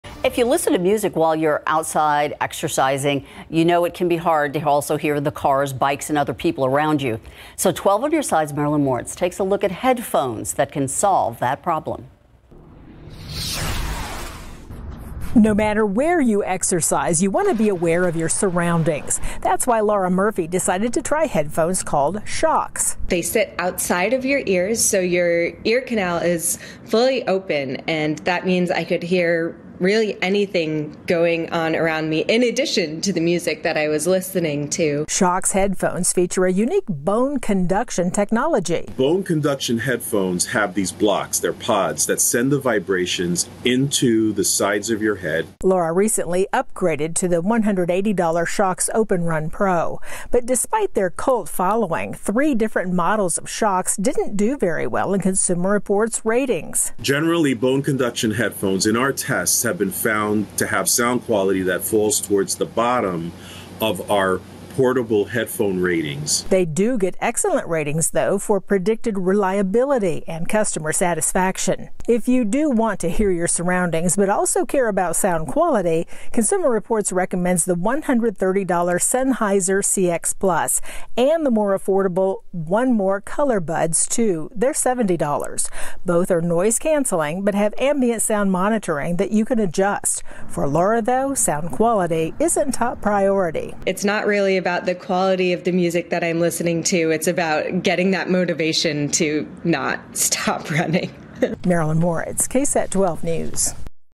Best headphones to use while exercising outdoors
If you listen to music while you exercise outdoors, you know how hard it can be to hear cars, bikes or other people around you. 12-On-Your-Side's Marilyn Moritz takes a look at headphones that can solve that problem.